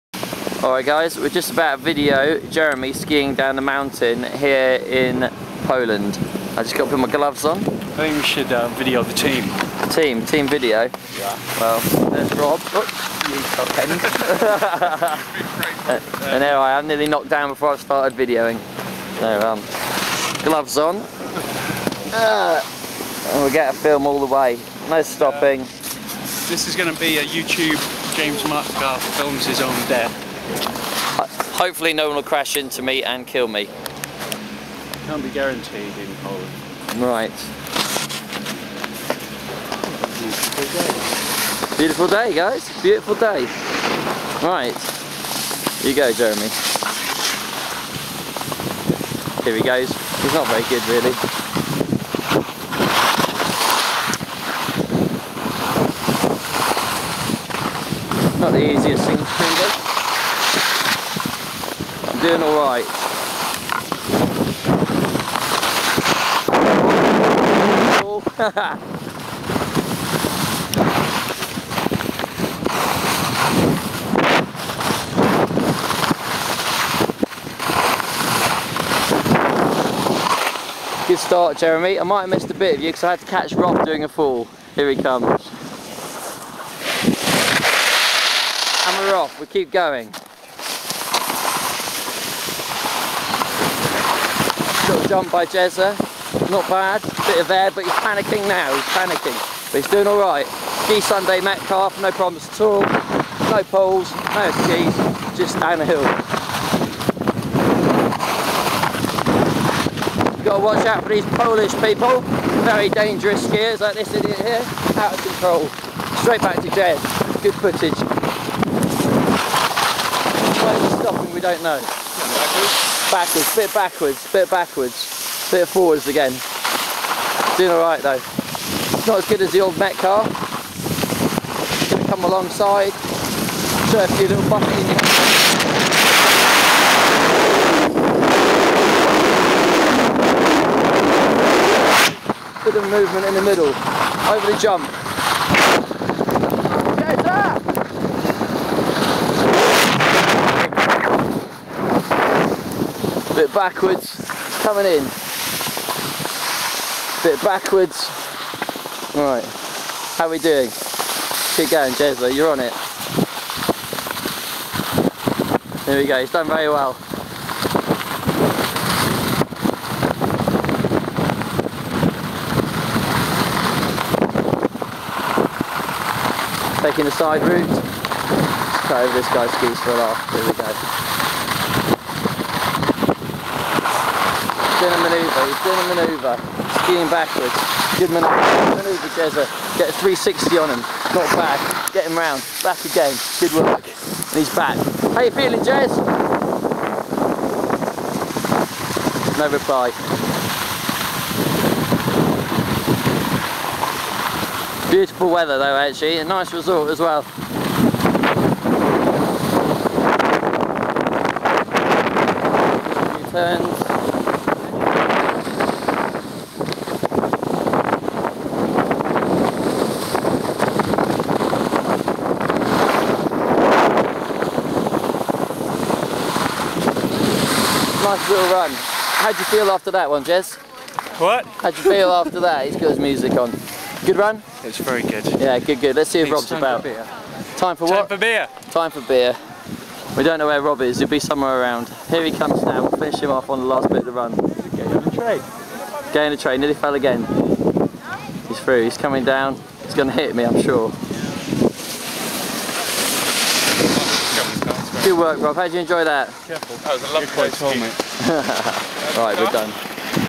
Jeremy Milligan Skiing in Poland filmed and narrated by James Metcalfe, extras: Rob Hodgkinson 2009
Skiing in Poland 2009